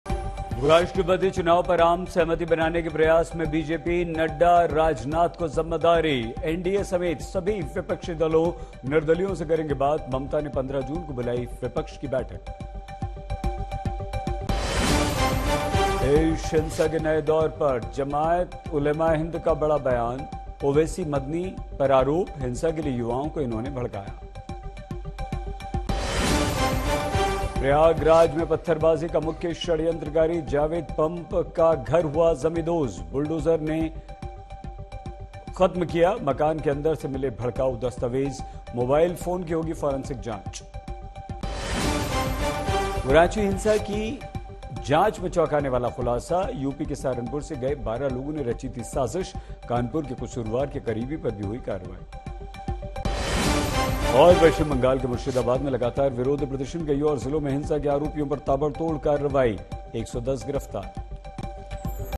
Hindi News Live: शाम 06:00 बजे की बड़ी खबरें | Headline | 12th June 2022 | Presidential elections
राष्ट्पति चुनाव पर आम सहमति बनाने की कोशिश में बीजेपी,जेपी नड्डा-राजनाथ को मिली जिम्मेदारी-NDA समेत सभी विपक्षी दलों और निर्दलीयो से करेंगे बात-ममता ने 15 जून को बुलाई विपक्षी बैठक

देश हिंसा के नए दौर पर जमाअत उलेमा ए हिंद का बड़ा बयान,ओवैसी और मदनी पर लगाया आरोप-हिंसा के लिए युवाओं को भड़काया

BJP, JP Nadda-Rajnath got the responsibility in an attempt to build a consensus on the Presidential election - will talk to all the opposition parties and independents including NDA - Mamata called the opposition meeting on June 15

Jamaat Ulema-e-Hind's big statement on the new era of country violence, accused Owaisi and Madani - incited youth for violence